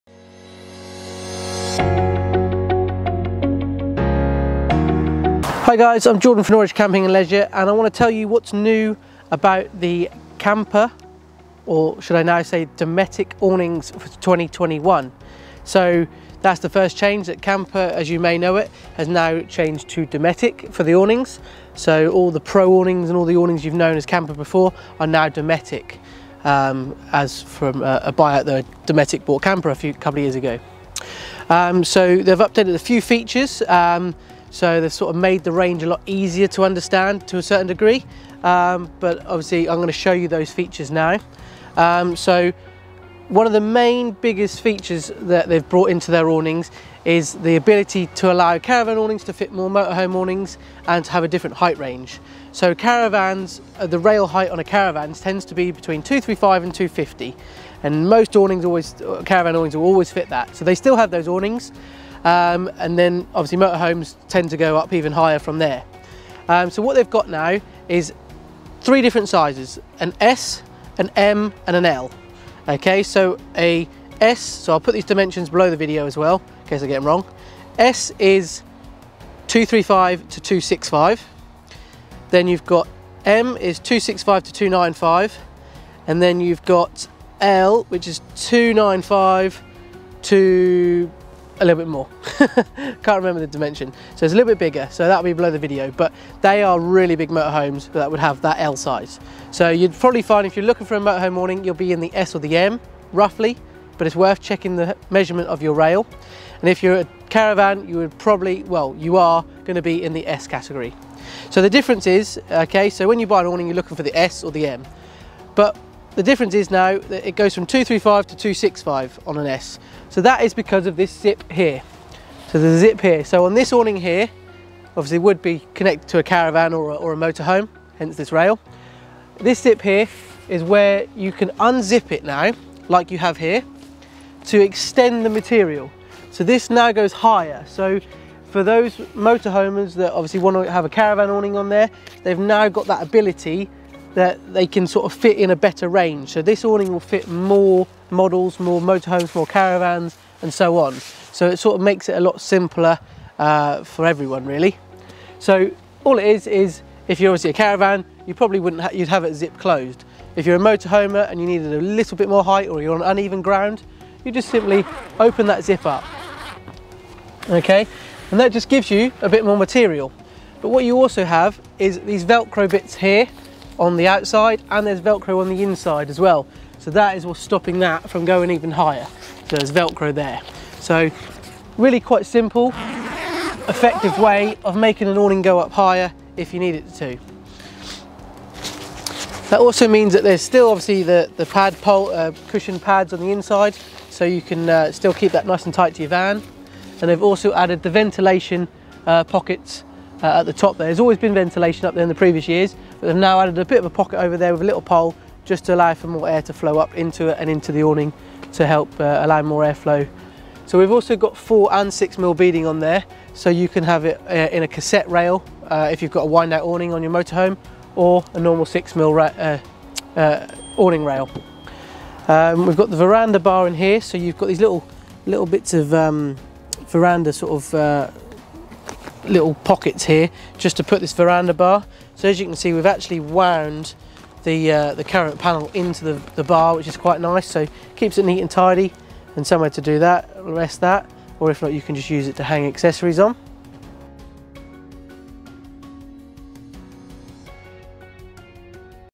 Kampa Dometic Updates | What's NEW for 2021
Great updates for 2021 from Dometic, previously known as Kampa,

Kampa still going as a brand for the budget range of tents and driveways

The new zip system allows for the awning to fit more motorhomes and caravans, dual beading for caravan rails and wideout cassette awnings

New size guide from Kampa / Dometic
S - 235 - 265cm (Caravans & Motorhomes)
M - 265 -295 cm (Motorhomes)
L - 295-325cm (Motorhomes Club models only)

Products
Caravan awnings